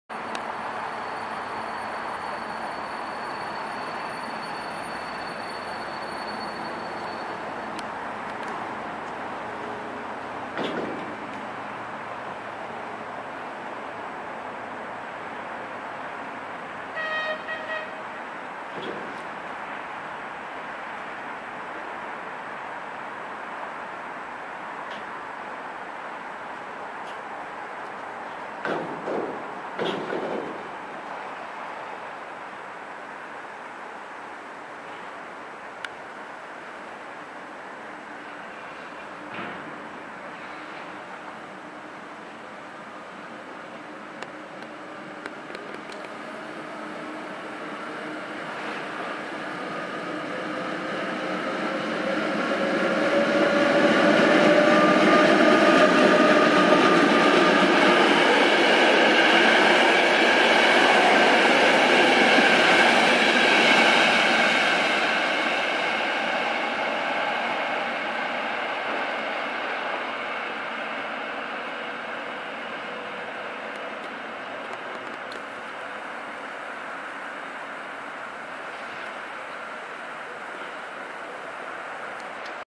Lokomotiva 363 022-5 - 23.1.2014
Po krátké pauze pokračuje lokomotiva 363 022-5 se soupravou měřicích vozů SŽDC ze stanice Zábřeh ve směru Olomouc. Natočeno ve čtvrtek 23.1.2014 v 10.26 hodin